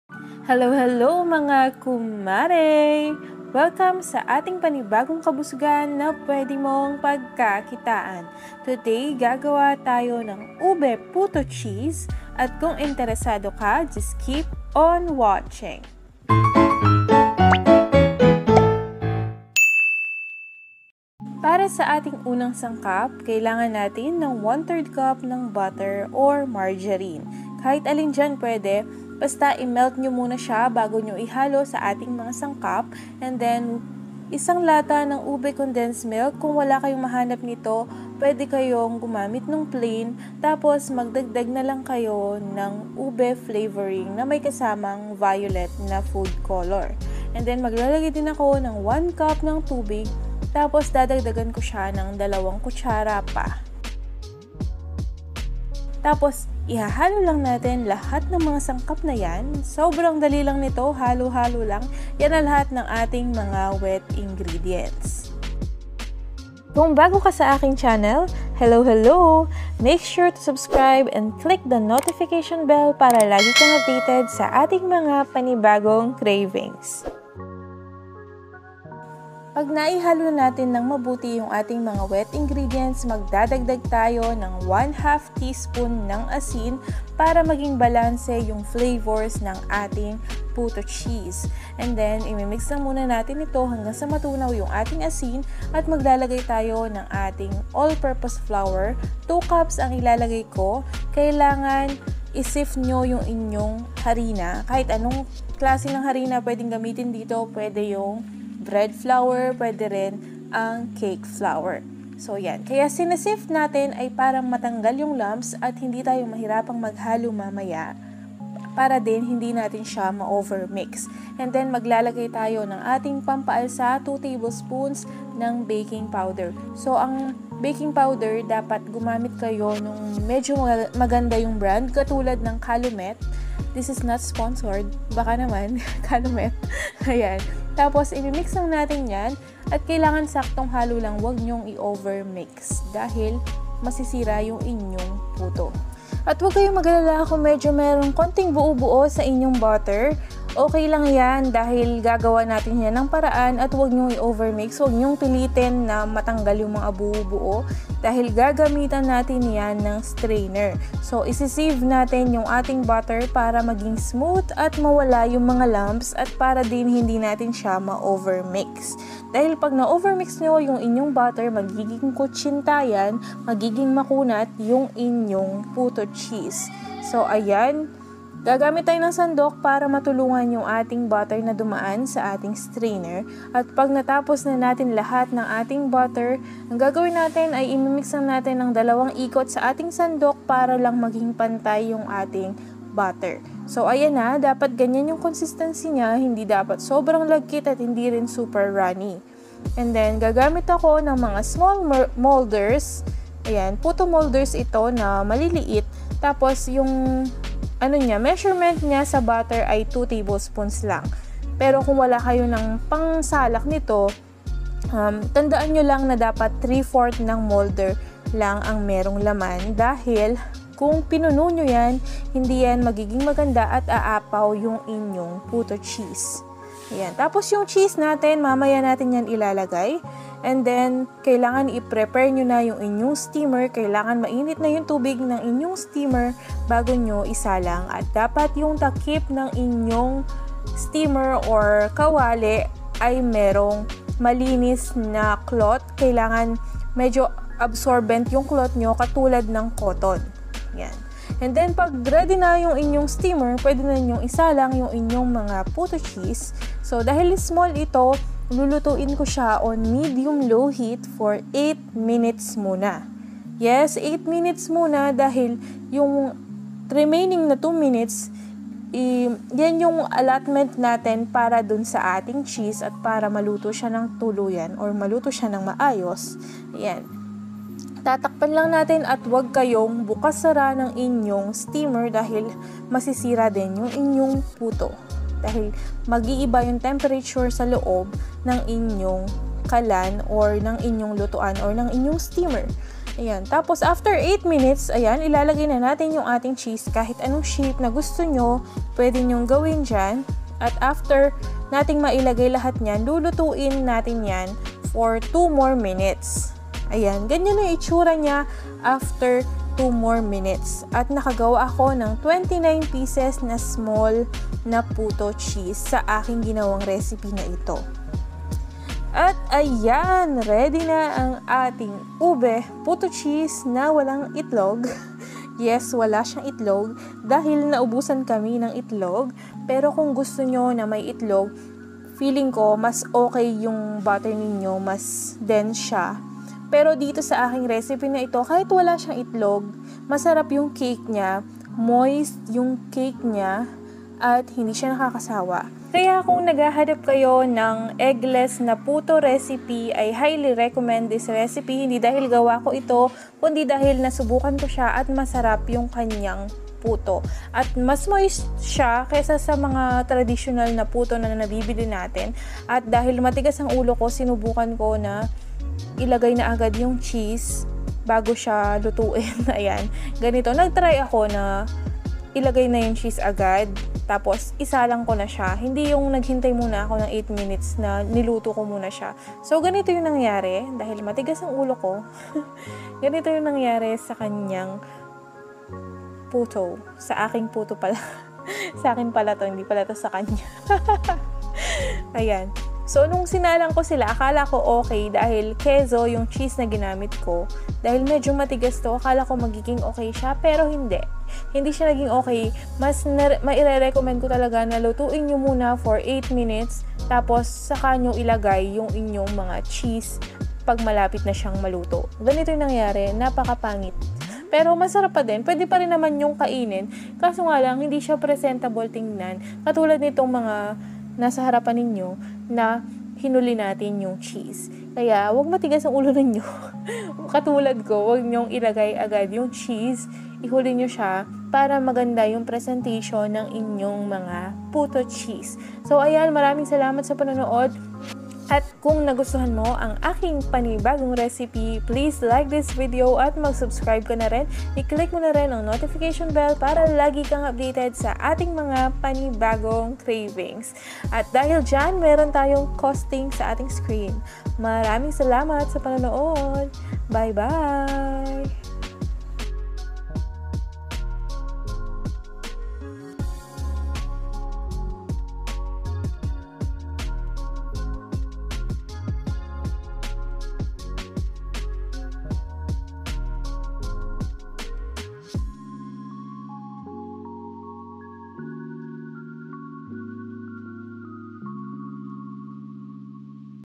Gawin mo ito sa 1/4 Kilo PUTOCHEESE NEGOSYO MALAMBOT AT MAUMBOK NA PUTONG WALANG ITLOG P90 PUHUNAN
1/4 Kilo PUTOCHEESE NEGOSYO MALAMBOT AT MAUMBOK NA PUTONG WALANG ITLOG PERO MAY UBE BE P90 PUHUNAN

Ube PutoCheese Recipe
Condensed Milk Cake Eggless
Steamed Condensed Milk Cake
No Bake Condensed Milk Cake
Condensed Milk Cake Without Oven

Ingredients:
1/3 cup Butter or Margarine - 10php
390g Ube Condensed Milk - 35php
1/2 tsp Salt - .25php
1/4 kilo or 2 cups All Purpose Flour - 12.5php
2 tbsp Baking Powder - 4php
Cheese - 15php
1 cup water + 2 tbsp

Gas & Utilities - 15php
Total: 81.75php
Yield: 29pcs Small
Cost per Yield: 2.8php

SRP: 5-6php